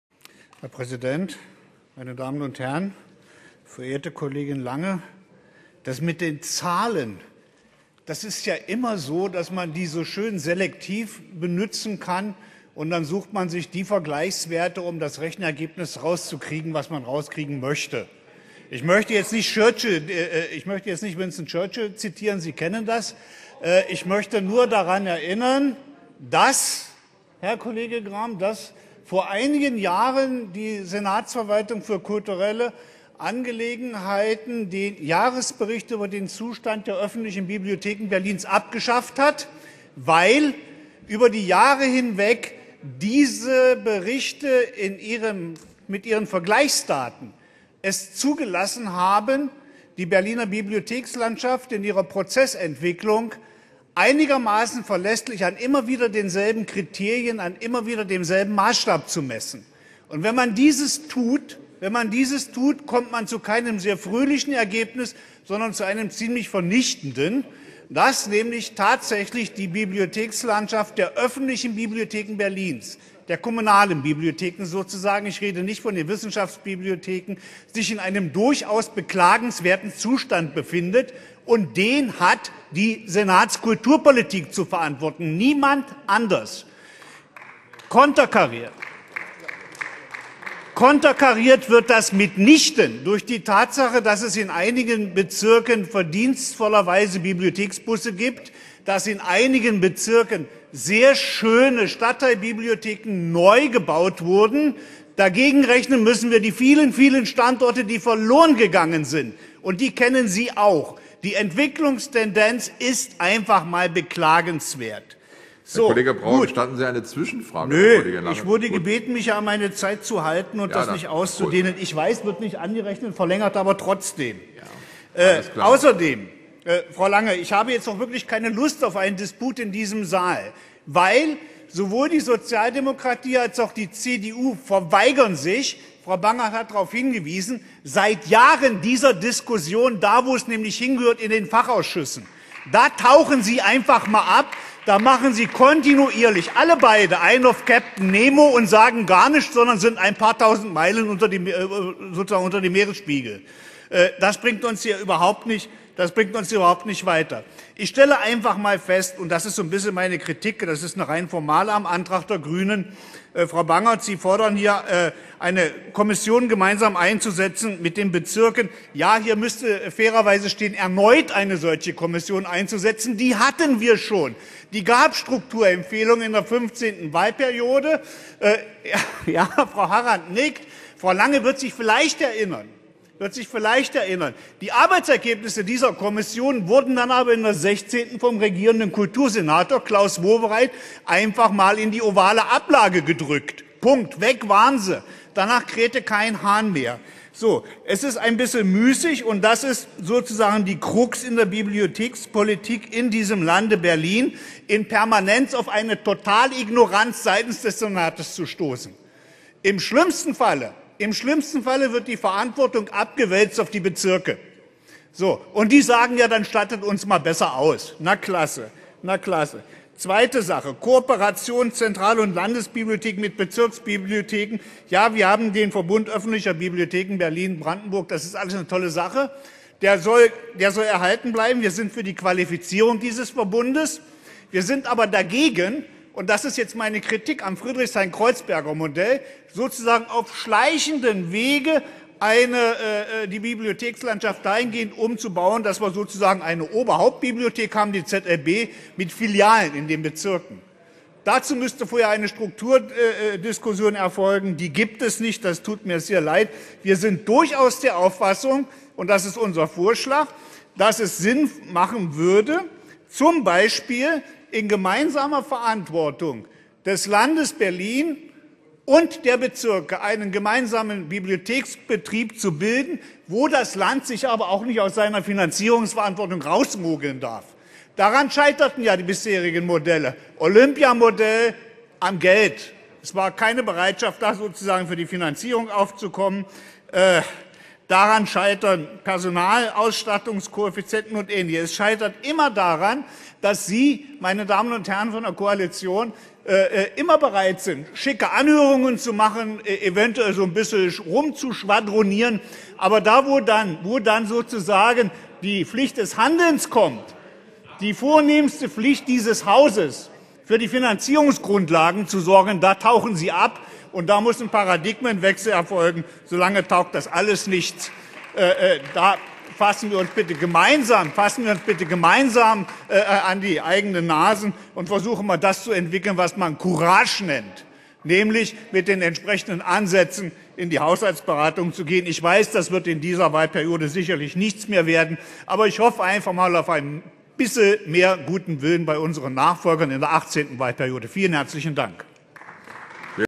66. Sitzung - Wolfgang Brauer zum Bibliothekskonzept für Berlin
11. Juni 2015
66. Sitzung des Berliner Abgeordnetenhauses
 Wolfgang Brauer (Die Linke)

sieht die öffentlichen, kommunalen Bibliotheken in Berlin in einem "beklagenswerten" Zustand. Zu verantworten habe dies die Senatskulturpolitik. Viele Standorte seien verloren gegangen. Die Verantwortung werde auf die Bezirke abgewälzt. Immer wenn es darum gehe, für die Finanzierungsgrundlagen zu sorgen, tauche die Koalition ab.